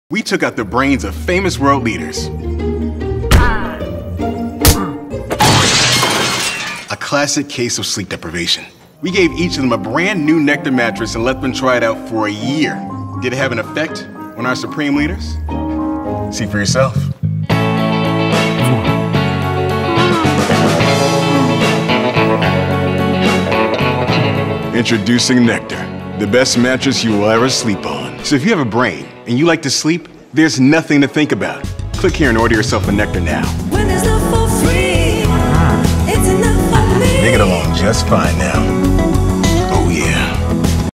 World Leaders Get Better Sleep With Nectar Mattress
Directed by - Noam Sharon & Tal Rosenthal (TSFM)
Head of creative - Amir Ariely- Google